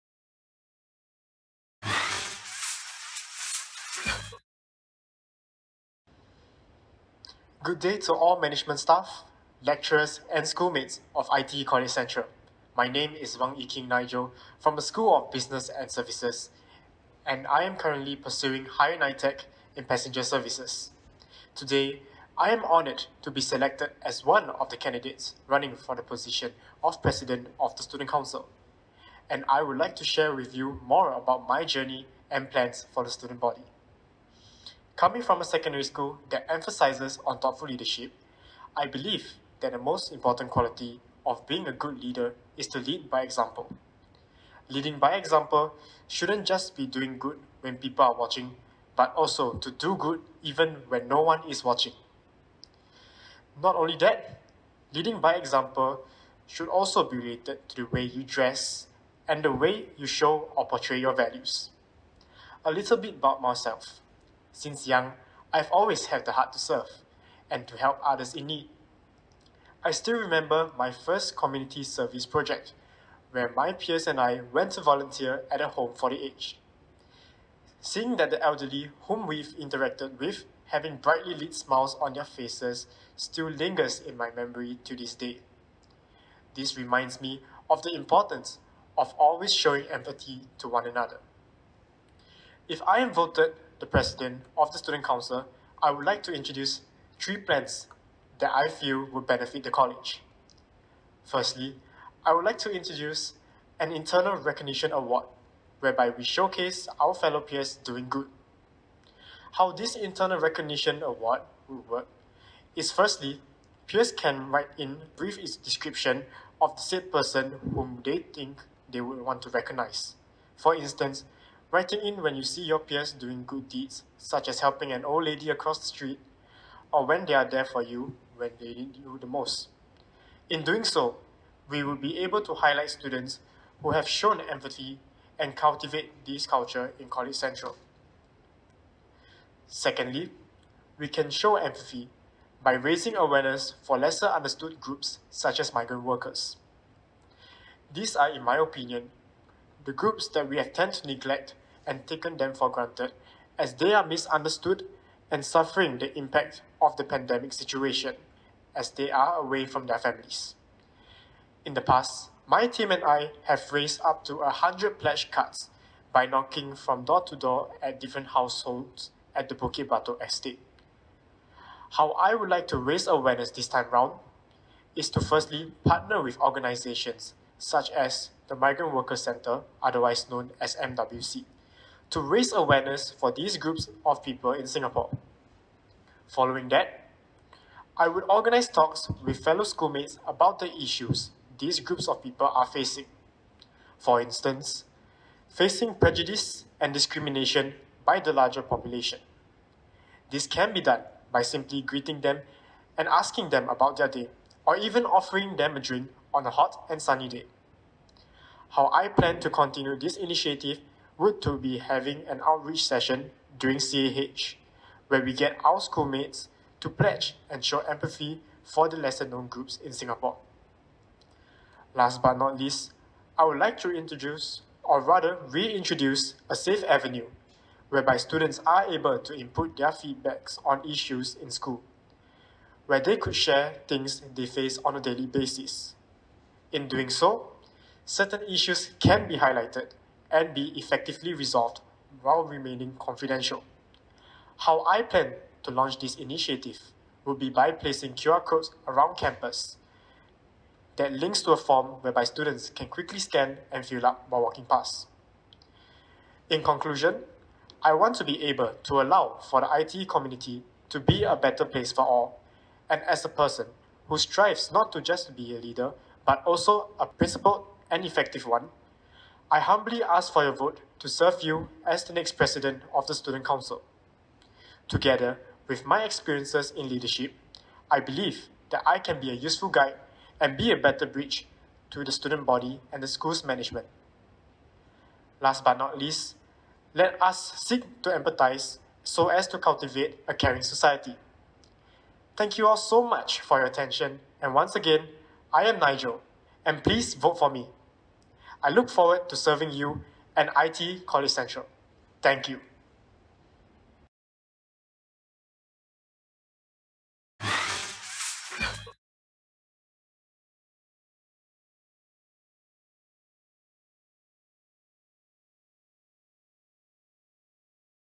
Nigel Wang - SC Presidential Speech 2021
WANG YEE KING NIGEL
Higher Nitec in Passenger Services (SBS)

Hear from our 3 Student Council Presidential elects as they share about their leadership values and also how they would contribute to the student body in the 'New Normal', if they are voted as the next SC President for 2021/2022.

More info from Voting link: TBC
(Voting opens: 2 to 5 Nov 2021)